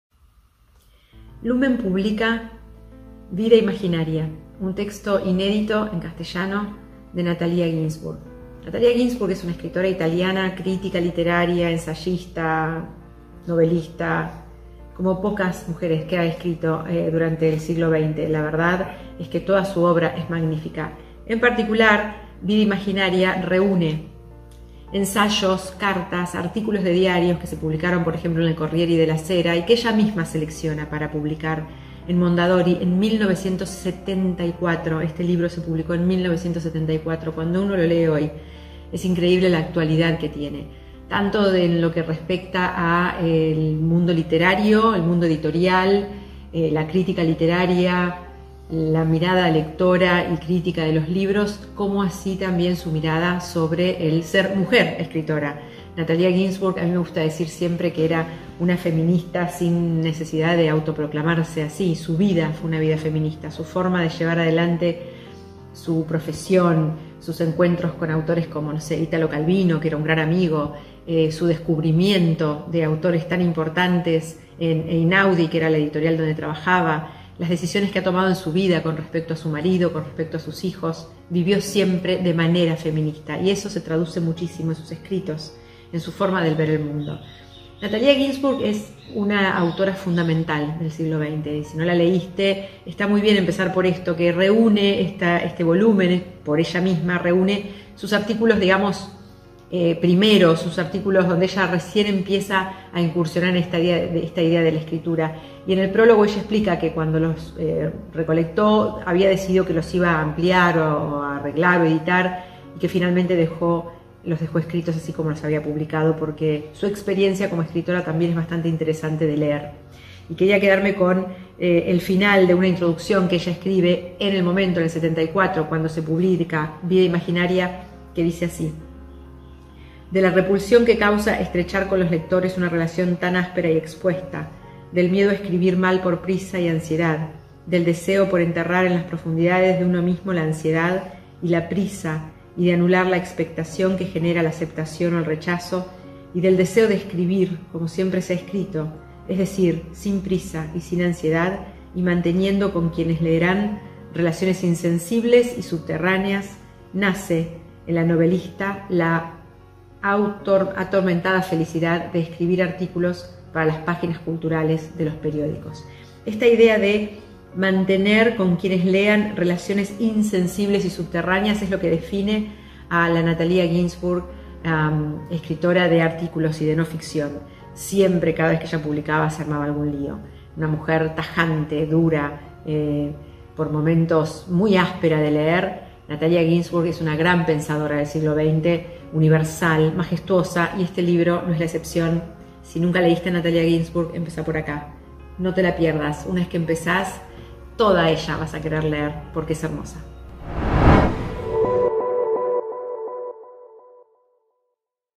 Un libro para recomendar: "Vida imaginaria", de Natalia Ginzburg
Traducidos por primera vez al español, estos artículos reunidos por una de las escritoras italianas fundamentales del siglo XX mantienen su vigencia a más de medio siglo de su publicación.